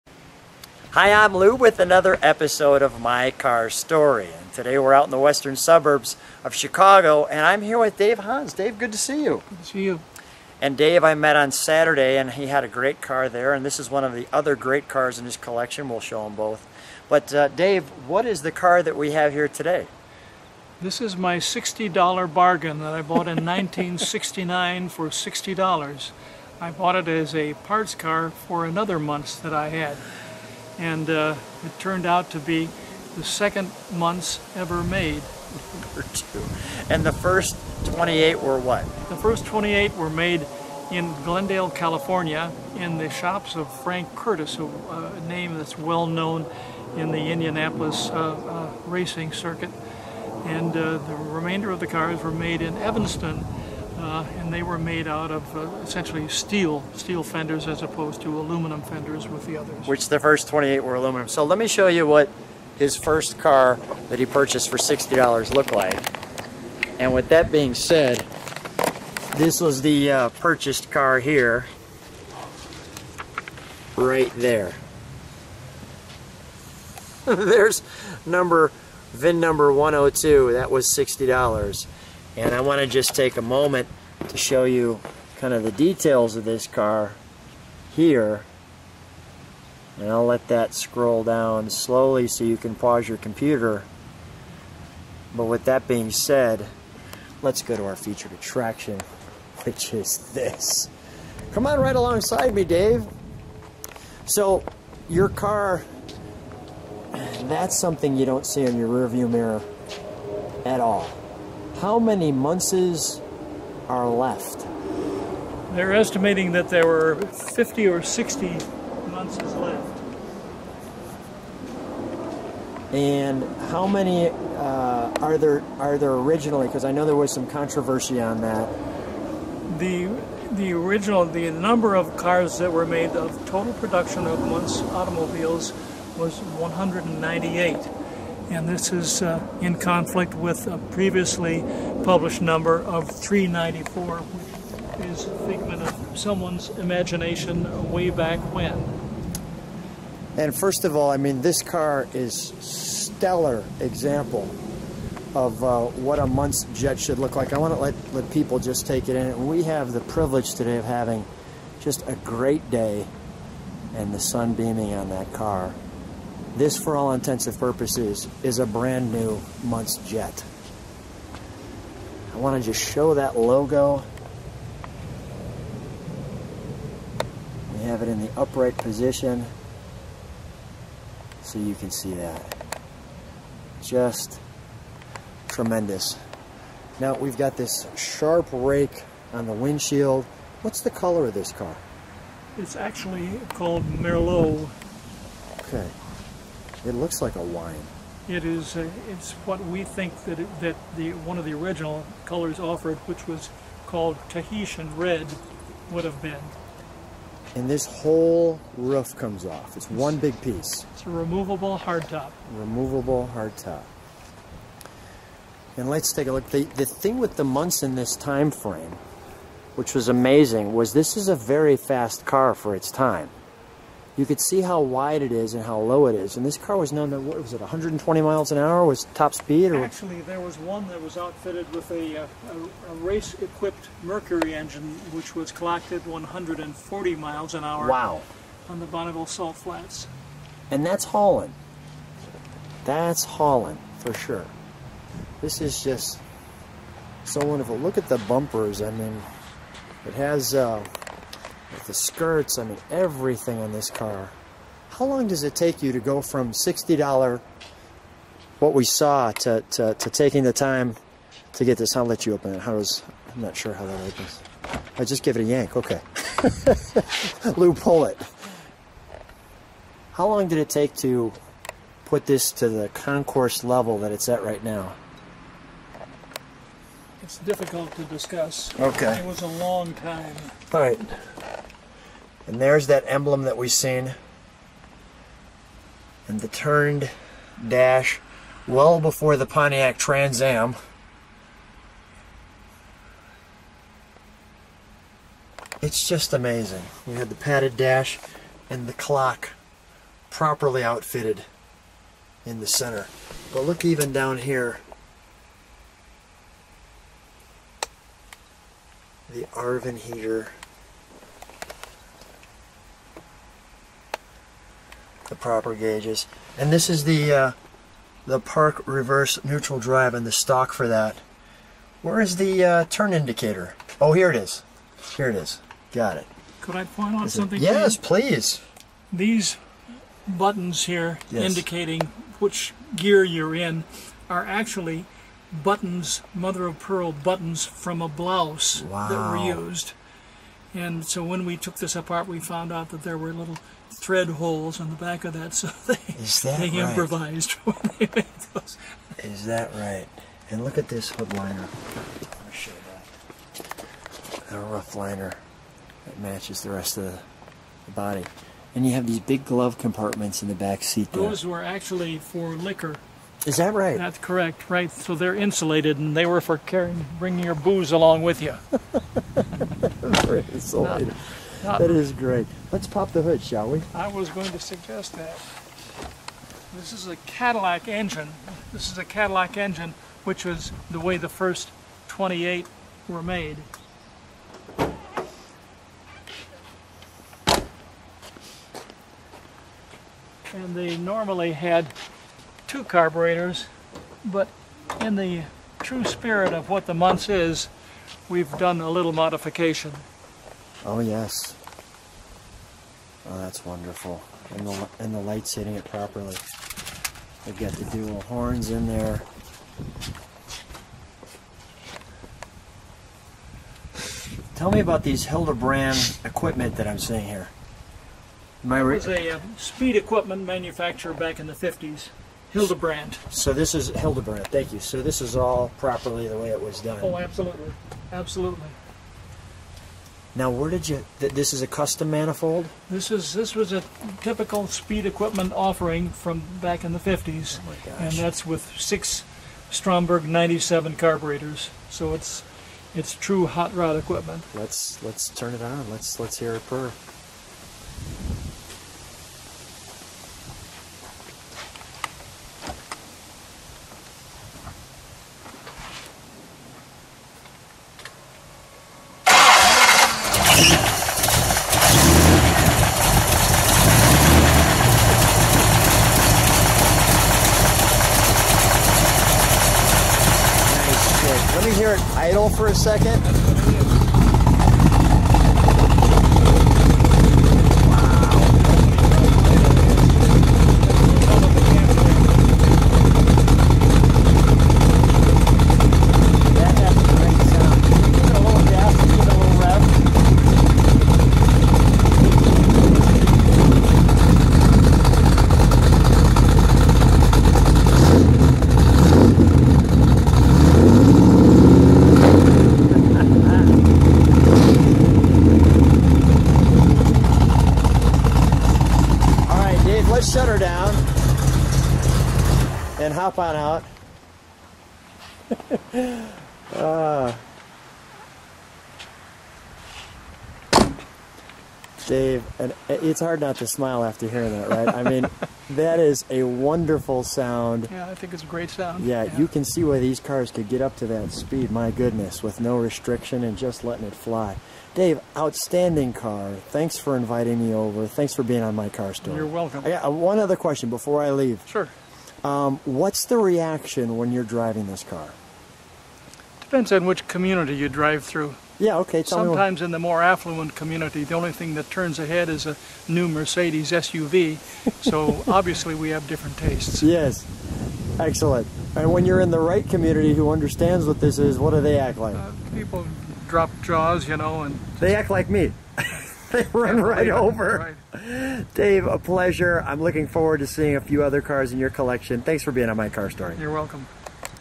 1950 Muntz Jet Convertible in Merlot Red Paint & Engine Sound on My Car Story with Lou Costabile
On "My Car Story" we're in the West Suburbs of Chicago on 6-5-17.

We're looking at a 1950 Muntz Jet Convertible (removable hardtop) in Merlot Red (to look like the original color Tahitian Red) Paint.

The car's Owner is Dave Hans.  Dave’s had this car since 1969.  He found the car in a junkyard and purchased it for $60.00.  His plan was to use the car as a parts car for another Muntz Jet.  Dave discovered he had the # 2 car produced, which of the first 28 cars built, all were built in California and made out of aluminum.  The cars built after the first 28, were built in Chicago and were built out of steel.

This restoration is stellar, and the car looks like it just came off the showroom floor.